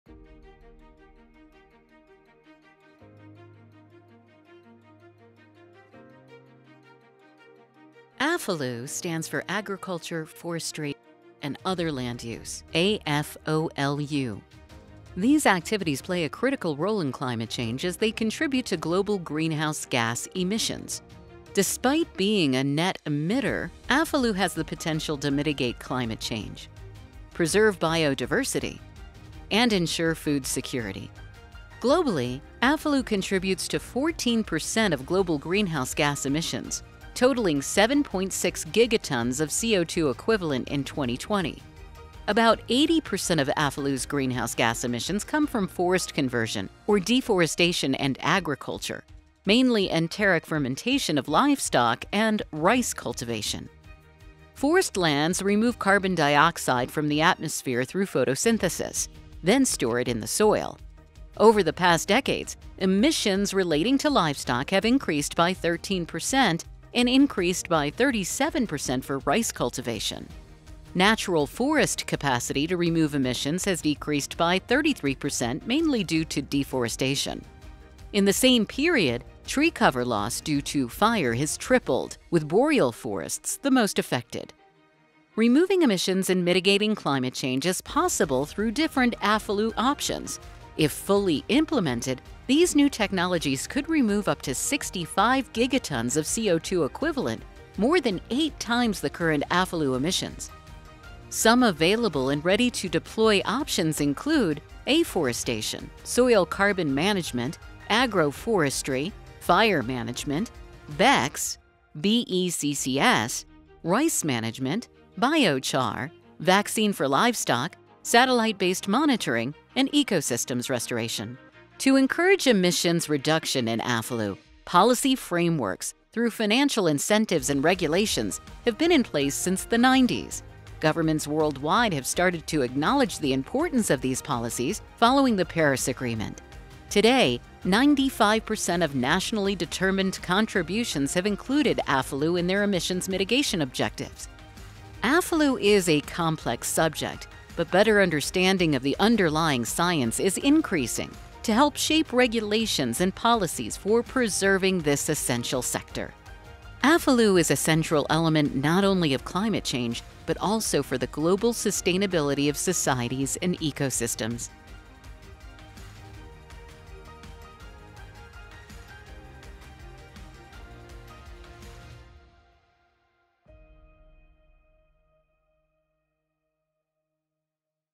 Agriculture, forestry, and other land use (AFOLU)
This latest FactBook from Kearney's Energy Transition Institute focuses on greenhouse gas (GHG) emissions (CO₂,CH₄ and N₂O) from Agriculture, Forestry
and Other Land Use (AFOLU) sector. It identifies key sources of emissions in the sector along-with major
trends. The factbook outlines key mitigations measures which offer an opportunity to reduce GHG
 emissions as well as enrich biodiversity and human well being.